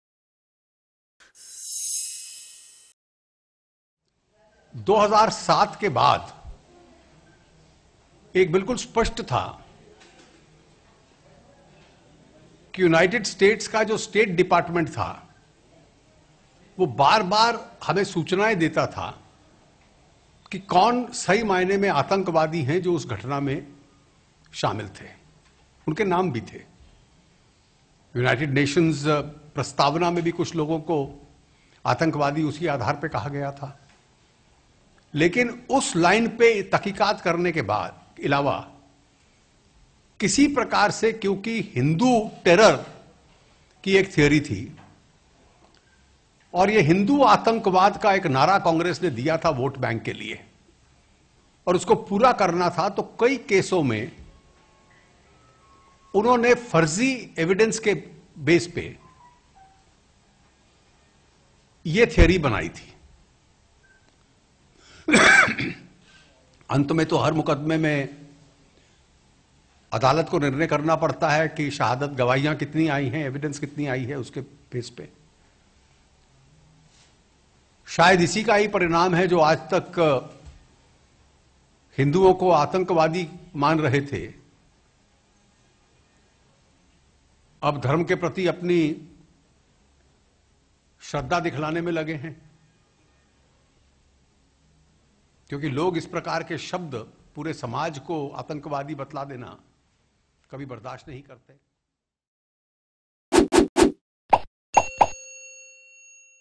समझौता ब्लास्ट फैसले के बाद Jaitly ने Congress को घेरा, Vote के लिए दिया 'हिंदू आतंकवाद' का नारा
समझौता ब्लास्ट बम धमाका मामले में स्वामी असीमानंद और तीन अन्य आरोपियों को बरी करने वाली एक अदालत द्वारा इस मामले में विश्वसनीय और स्वीकार्य साक्ष्य की कमी की टिप्पणी के बाद बीजेपी ने कांग्रेस पर बड़ा हमला बोला है। बीजेपी ने आज आरोप लगाया कि कांग्रेस ने जानबूझकर हिंदू आतंकवाद शब्द का यूज किया और वोट के लिए हिंदू आतंकवाद का नारा दिया। गौरतलब है कि अदालत ने समझौता ब्लास्ट मामले में चारों आरोपियों- स्वामी असीमानंद, लोकेश शर्मा, कमल चौहान और राजिंदर चौधरी को 20 मार्च को बरी कर दिया था।